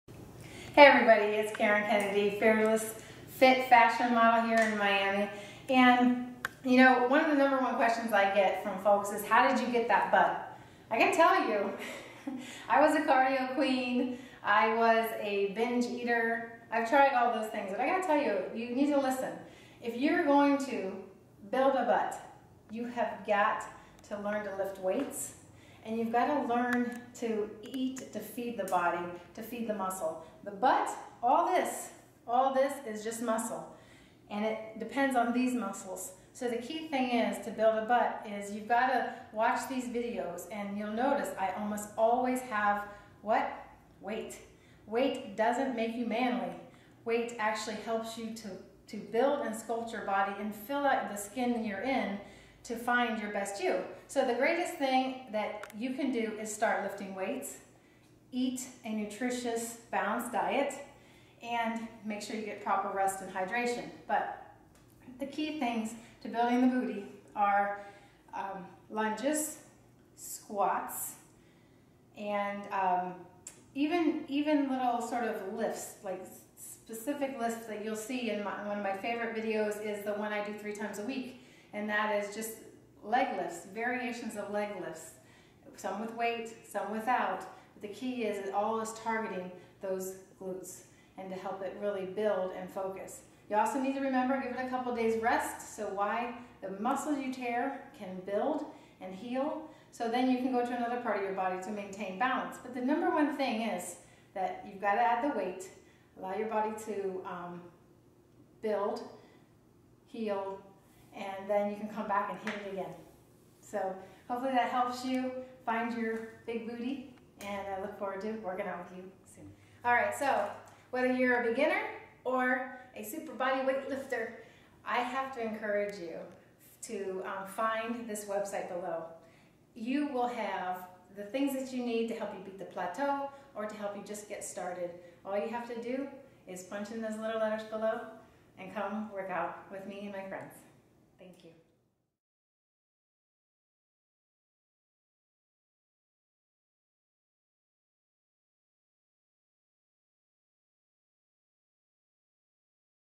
How to Get a Big Butt Naturally (Fast Results See Link In Description)
Fitness Model Karen Kennedy gives advice on how to get a lifted big round booty! If you want a round lifted butt you need to lift weights with the correct exercises and eat enough calories.
For the Best Butt Workout visit: -Get Paid To Look Fantastic FAST!